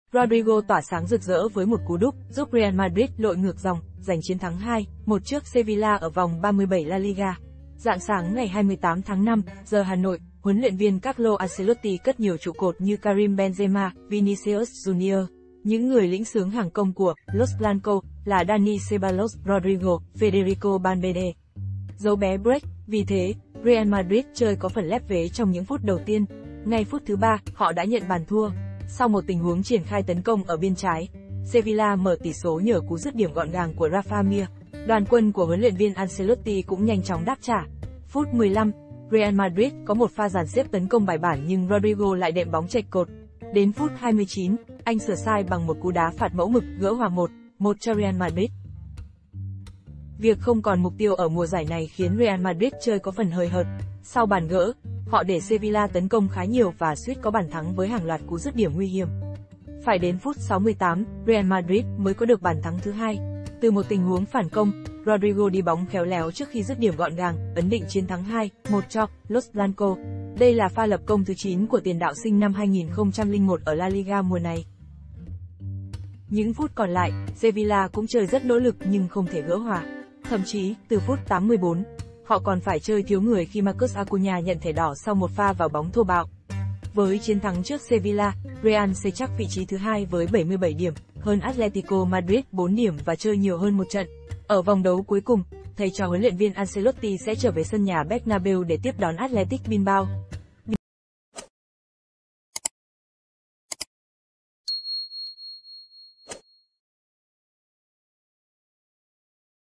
SAO TRẺ BRAZIL GIÚP REAL NGƯỢC DÒNG TRƯỚC SEVILLA
Rodrygo tỏa sáng rực rỡ với một cú đúp, giúp Real Madrid lội ngược dòng, giành chiến thắng 2-1 trước Sevilla ở vòng 37 La Liga. Rạng sáng ngày 28 tháng 5 (giờ Hà Nội), huấn luyện viên Carlo Ancelotti cất nhiều trụ cột như Karim Benzema, Vinicius Junior. Những người lĩnh xướng hàng công của "Los Blancos" là Dani Ceballos, Rodrigo, Federico Valverde.